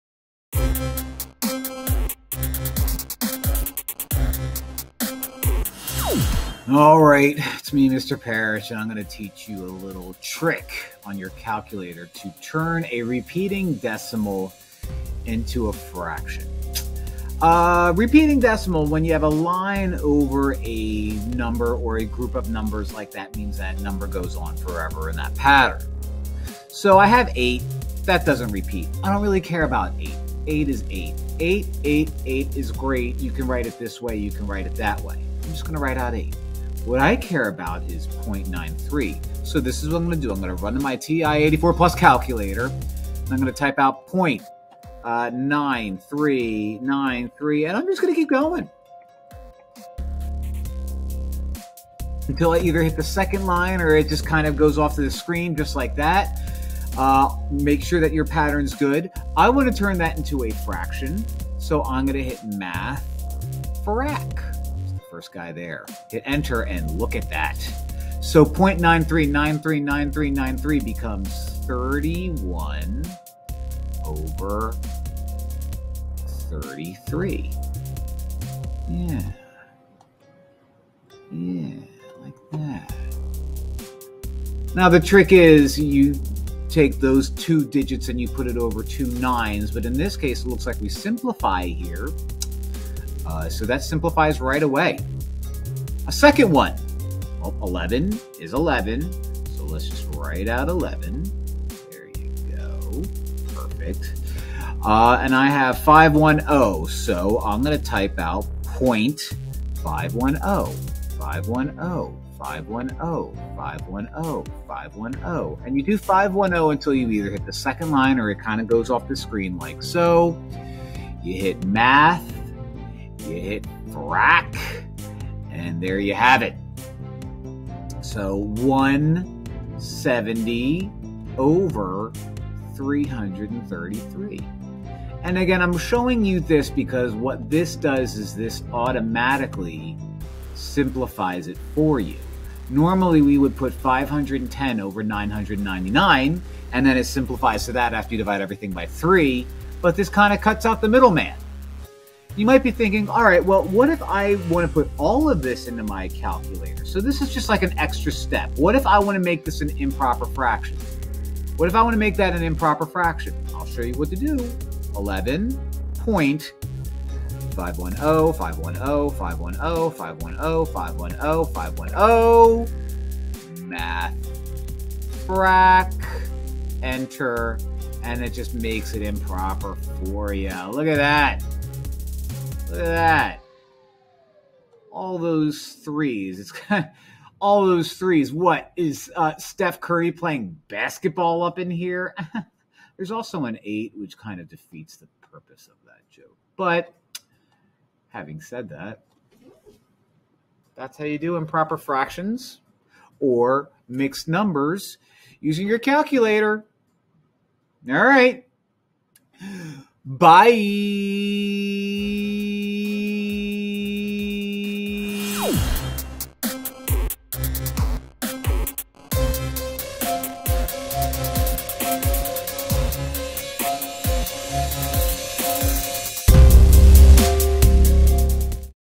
Calculator Tricks - Convert Repeating Decimals into Fractions￼ using a TI-84 Plus Calculator
On this episode of Calculator Tricks, I show how to graph Repeating decimals into fractions, both mixed fractions and improper fractions, on a￼ TI-84+ calculator.

Nick Perich
Stewart Middle School
Norristown Area School District
Norristown, Pa .